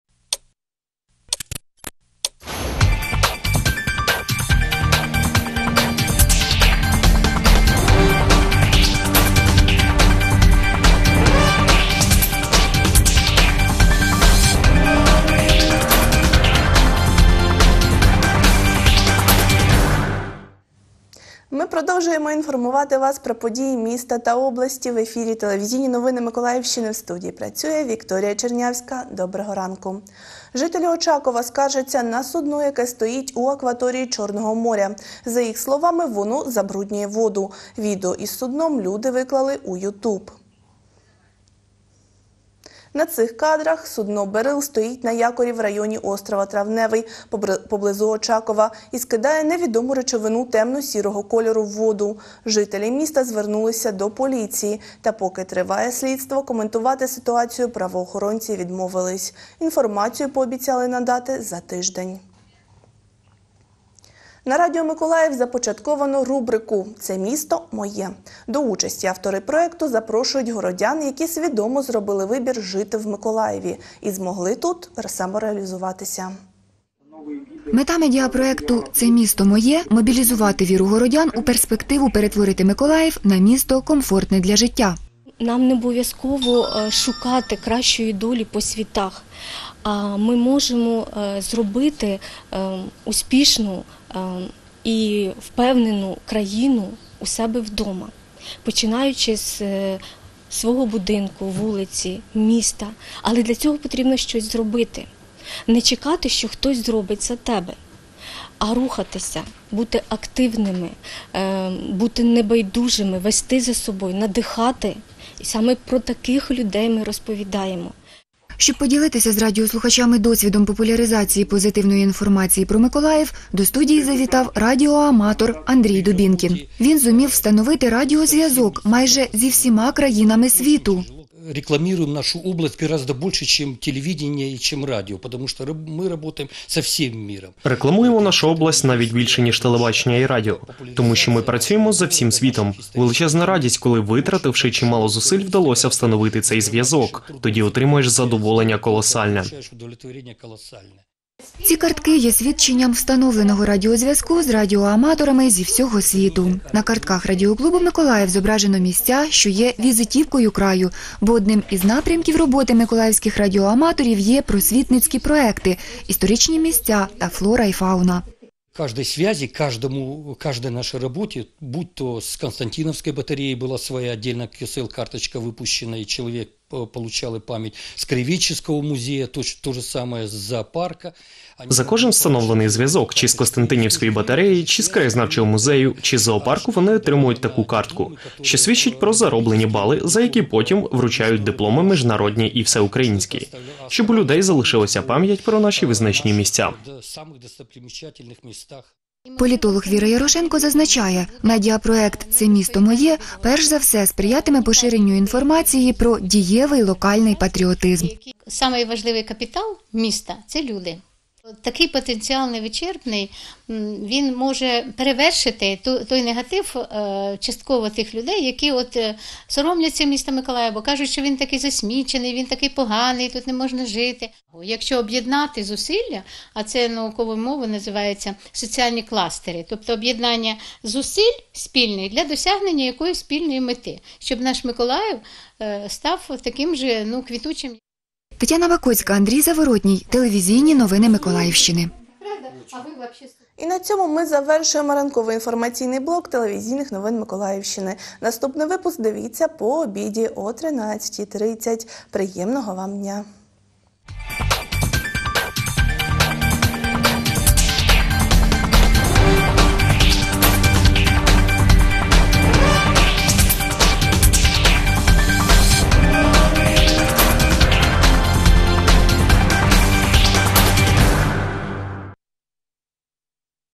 25.07.2018. Новини. 08:30
Інформаційна картина дня (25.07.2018) поглядом кореспондентів телеканалу “Миколаїв” (випуск новин — 08:30):
- жителі  Очакова скаржаться на судно, яке стоїть на якорі в районі о. Травневий;
- на радіо «Миколаїв» започатковано рубрику «Це місто моє».